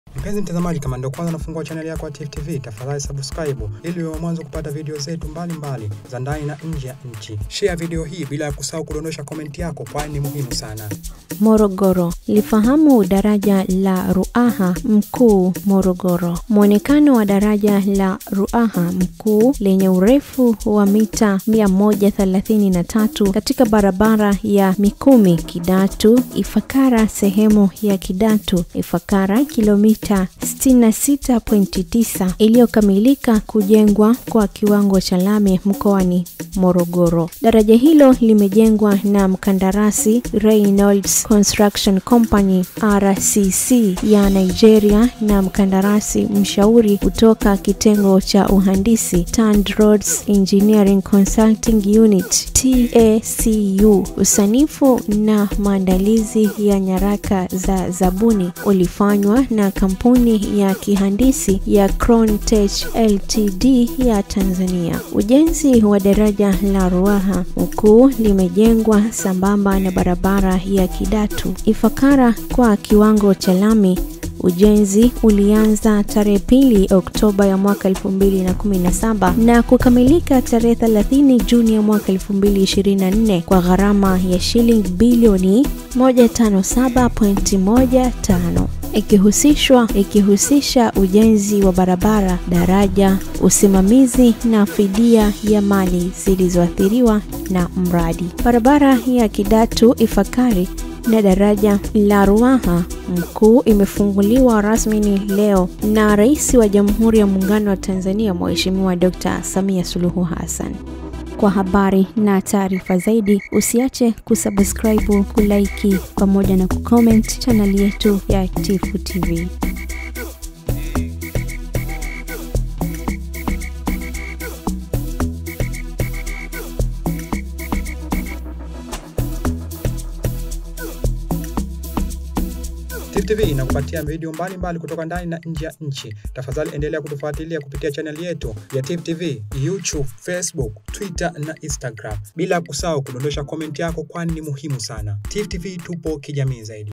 SIO NDOTO TENA NYARAKA ZA ZABUNI UJENZI UMEKAMILIKA HAYAWI HAYAWI MWISHO HUWA........
Morogoro

▪️LIFAHAMU DARAJA LA RUAHA MKUU - MOROGORO

Muonekano wa Daraja la Ruaha Mkuu lenye urefu wa mita 133 katika barabara ya Mikumi- Kidatu - Ifakara sehemu ya Kidatu - Ifakara kilometa 66.9 iliyokamilika kujengwa kwa kiwango cha lami Mkoani Morogoro.

Daraja hilo limejengwa na Mkandarasi Reynolds Construction Company - RCC ya Nigeria na Mhandisi Mshauri kutoka Kitengo cha Uhandisi (TANROADS Engineering Consulting Unit - TECU).

Usanifu na maandalizi ya nyaraka za zabuni ulifanywa na Kampuni ya kihandisi ya Crown-Tech Ltd ya Tanzania.

Ujenzi wa Daraja la Ruaha Mkuu limejengwa sambamba na barabara ya Kidatu - Ifakara kwa kiwango cha lami, Ujenzi ulianza 02 Oktoba 2017 na kukamilika 30 Juni, 2024 kwa gharama ya Shilingi Billion 157.15, ikihusisha ujenzi wa barabara, daraja, usimamizi na fidia ya mali zilizoathiriwa na mradi.

Barabara ya Kidatu - Ifakara na Daraja la Ruaha Mkuu imefunguliwa rasmi leo Agosti 04, 2024 na Rais wa Jamhuri ya Muungano wa Tanzania, Mheshimiwa Dkt. Samia Suluhu Hassan.

📸:W/Ujenzi